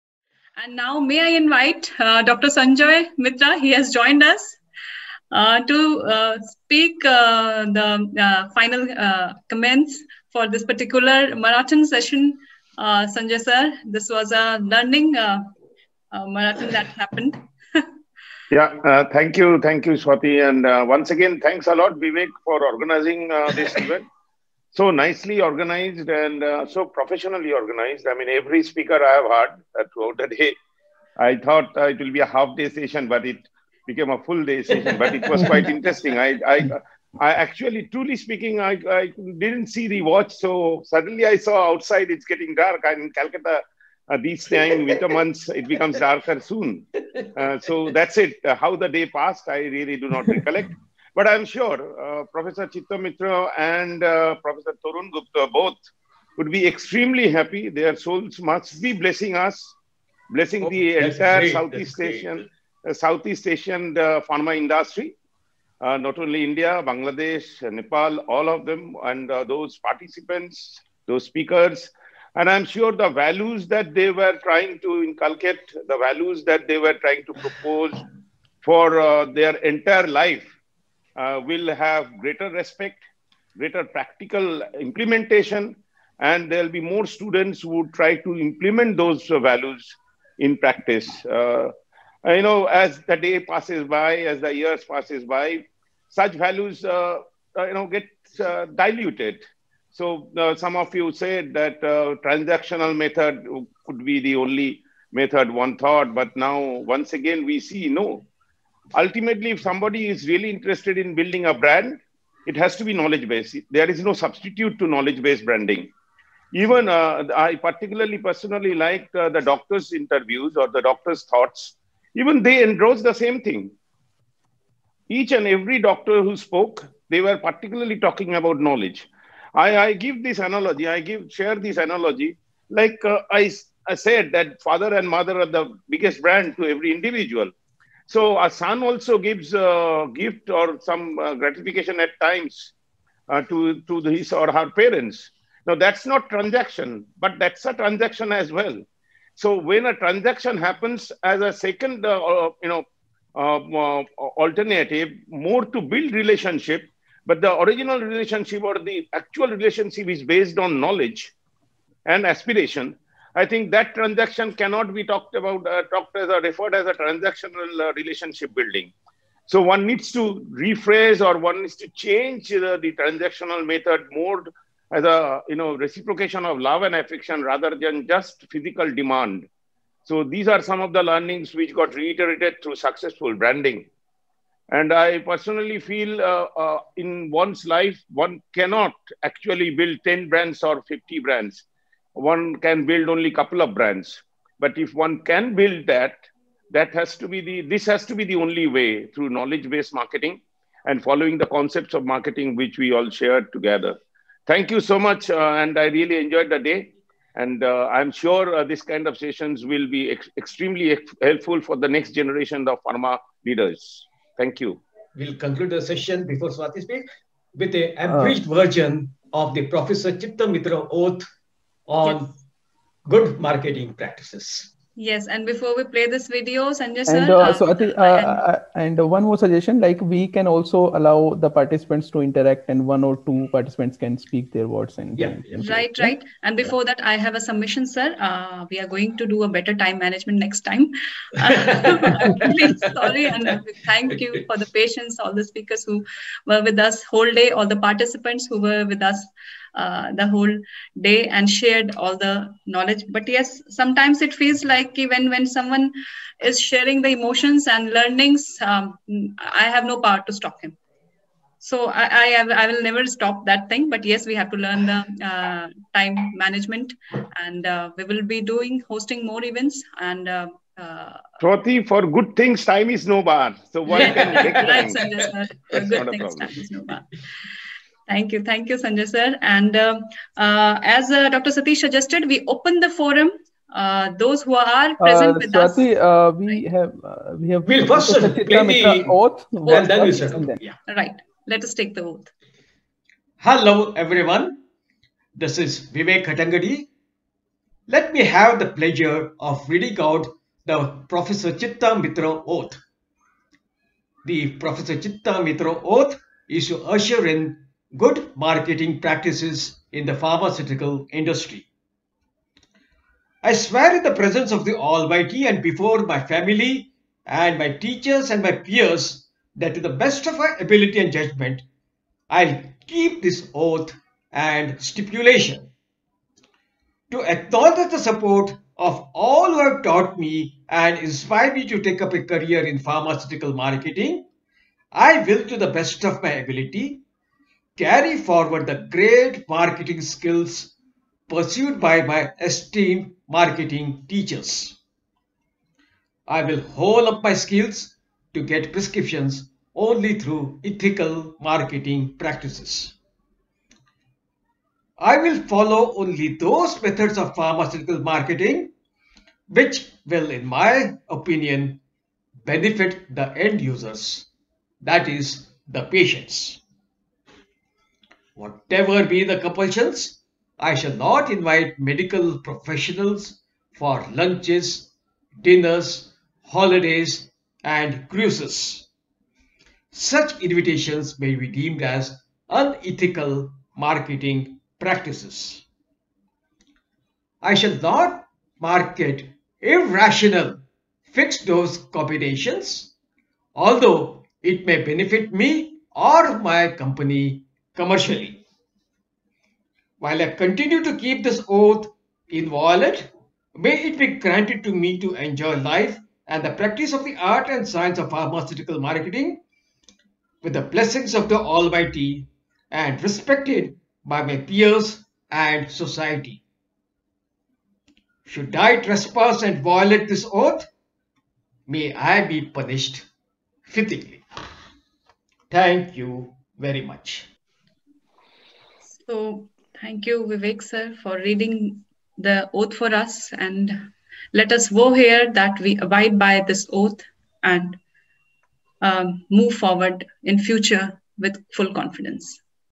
Part 19 - Dr. Sanjoy Mitra words of wisdom & Prof. Chitta Mitra Oath by Vivek Hattangadi
In this part, Dr. Sanjoy Mitra shares words of wisdom for Pharma Brand Management Enthusiasts.
Vivek Hattangadi & all participants of the event take The Prof. Chitta Mitra Oath to maintain the sanctity of the Pharma Brand Management Profession.